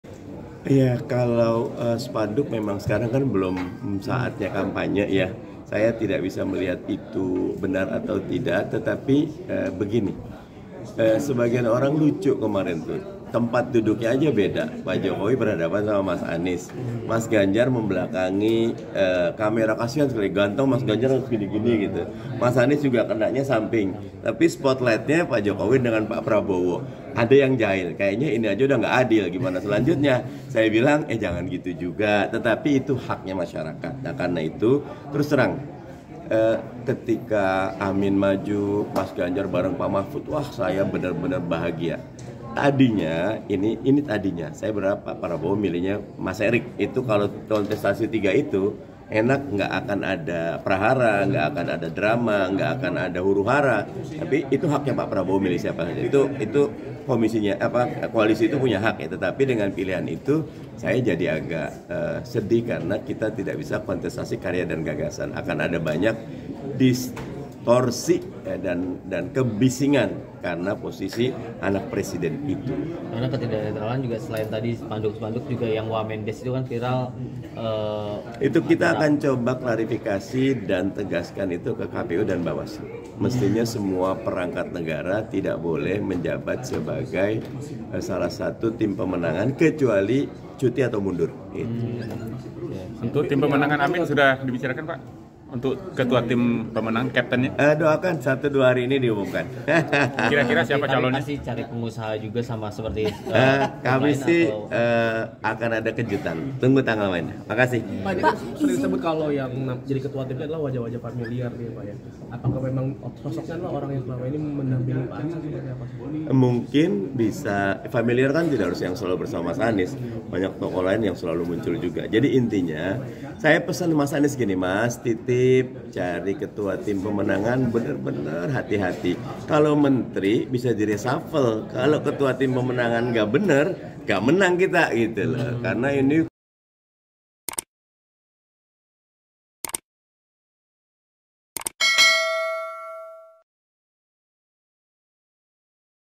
PKS Soroti Letak Duduk Anies Saat Jokowi Ajak 3 Bacapres Makan Siang
Ketua DPP PKS Mardani Ali Sera bicara perihal sorotan masyarakat terhadap posisi duduk tiga bacapres saat diundang makan siang oleh Presiden Jokowi.

Menurut Mardani, banyak pihak yang berusaha mengaitkan posisi duduk itu dengan arah dukungan Jokowi.

Hal ini disampaikan oleh Mardani saat ditemui di kompleks DPR, Senayan, Jakarta pada Selasa (31/10/2023).

"Spotlight-nya Pak Jokowi dengan Pak Prabowo, ada yang jail kayaknya ini saja sudah enggak adil. Ya saya bilang jangan begitu" kata Mardani.

Penulis Naskah: Michaela Winda Saputra
Video Jurnalis: Michaela Winda Saputra
Video Editor: Michaela Winda Saputra
Produser: Okky Mahdi Yasser